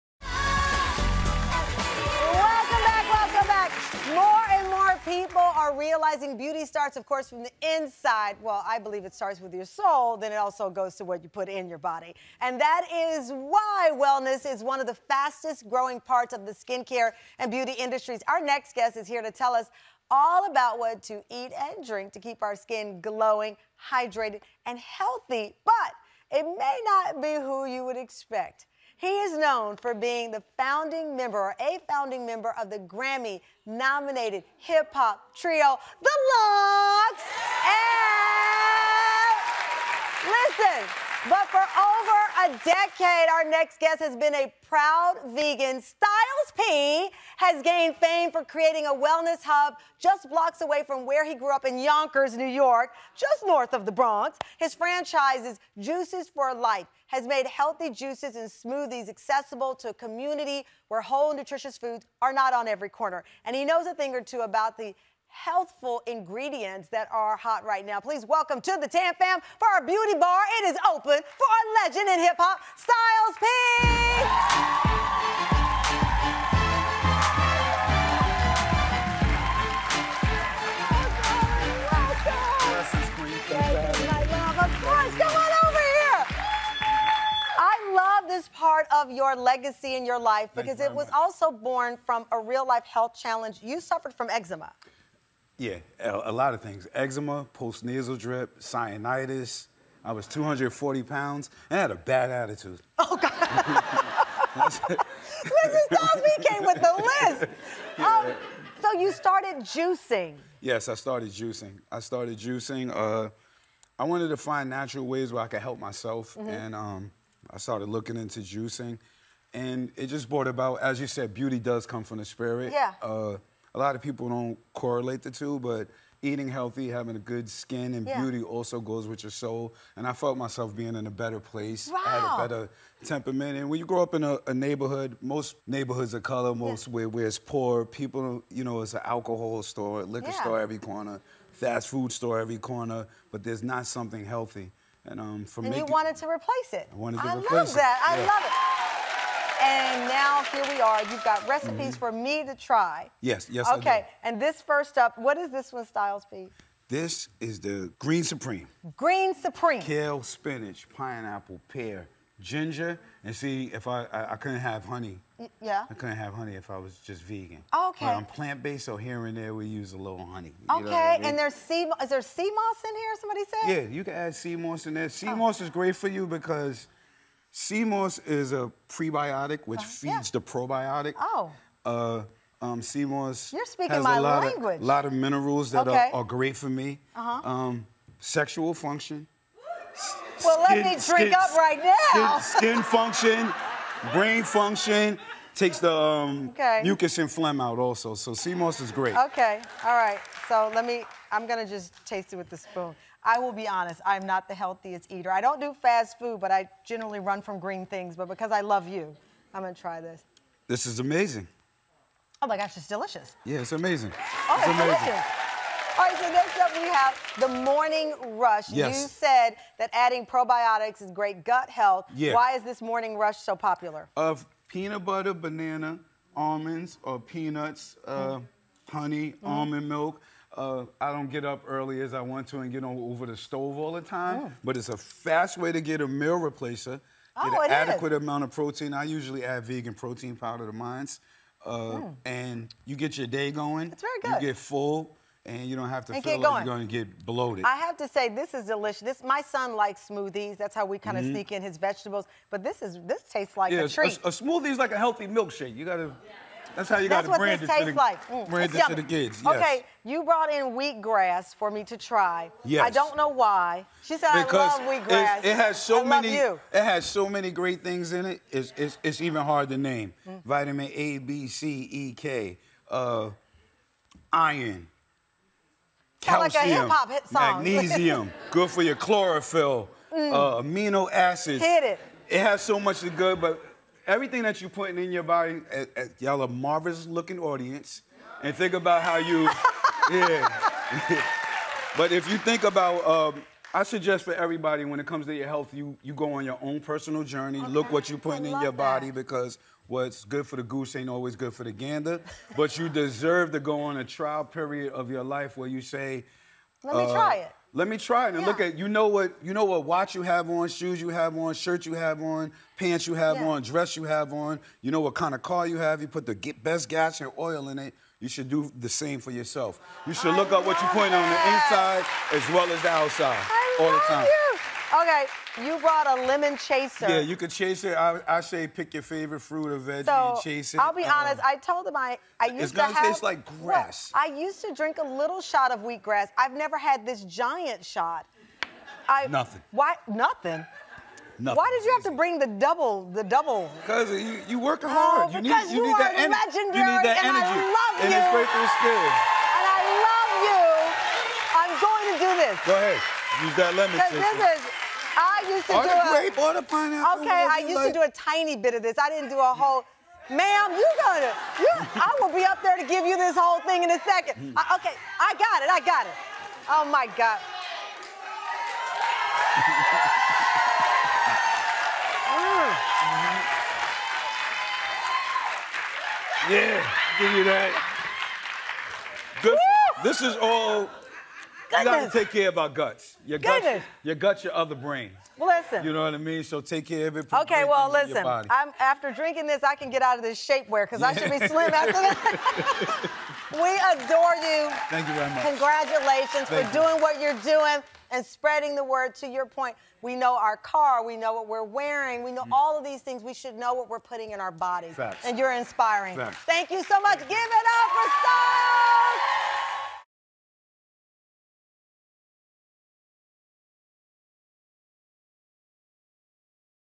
Styles P. Talks Eating Planted-Based For 10+ Years & the Benefits of Juicing
Hip hop icon and co-founder of the rap trio the Lox, Styles P, stops by our fall beauty bar! He tells the TamFam about his plant-based lifestyle for over a decade and being the co-owner of Juices For Life—a health bar franchise with two locations in New York. From the benefits of juicing to some of his favorite recipes, Styles doesn’t hold back. TamFam, you’ll be watching this one from start to finish!